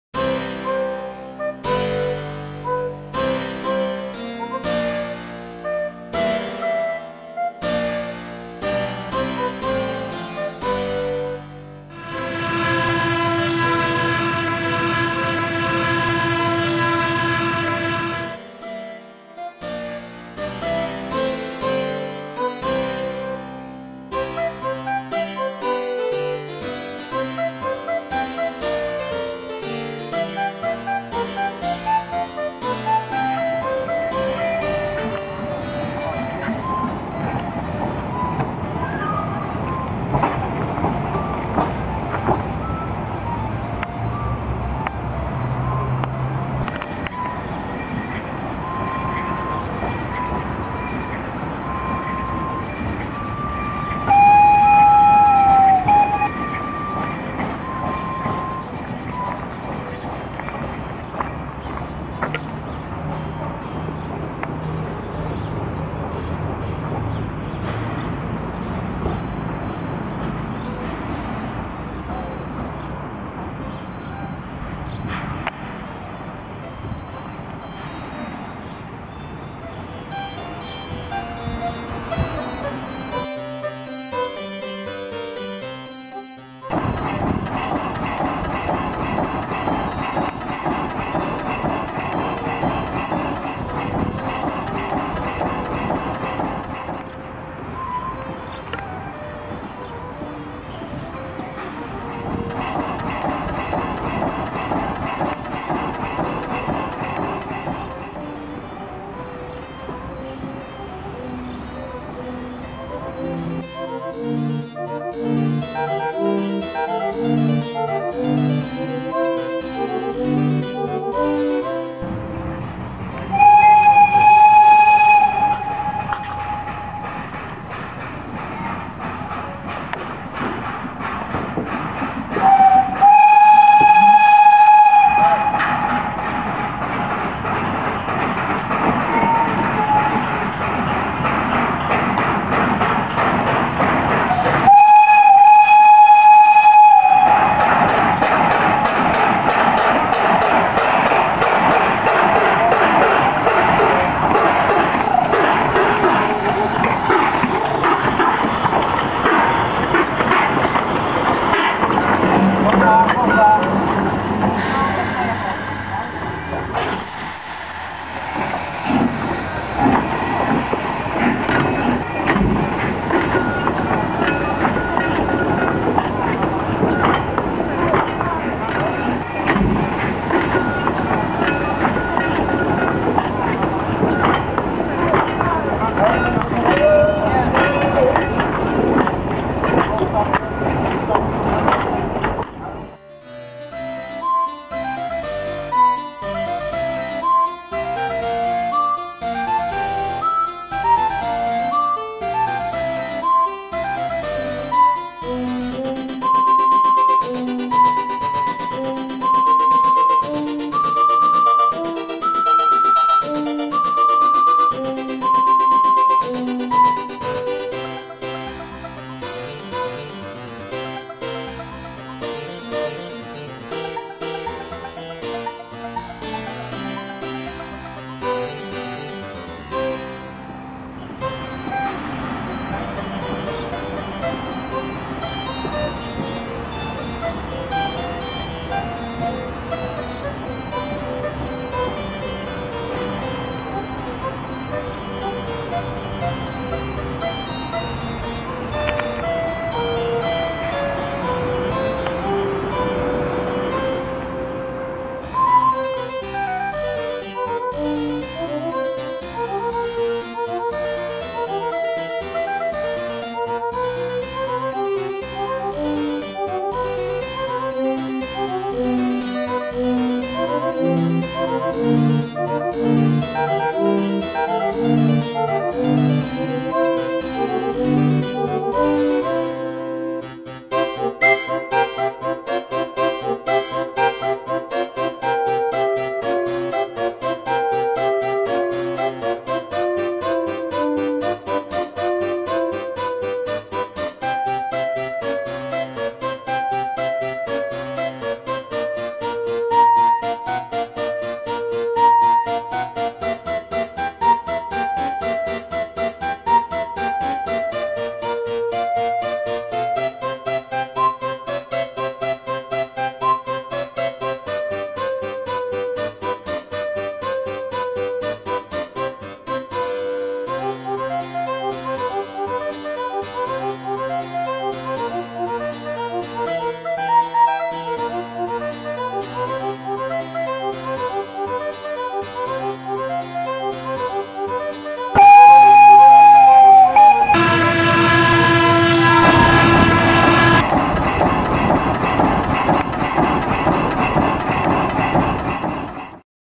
Kolej wąskotorowa - ROGOWO koło Łodzi
Kolej wąskotorowa łącząca Rogów z Rawą Mazowiecką i Białą Rawską powstała w 1915 roku podczas pierwszej wojny światowej, jako wojskowa kolej polowa zaopatrująca front w żywność broń i amunicję. Po odzyskaniu przez Polskę niepodległości została wcielona w struktury nowo powstałych Polskich Kolei Państwowych w ramach których funkcjonowała do 2001 roku, kiedy to podjęto decyzję o jej likwidacji.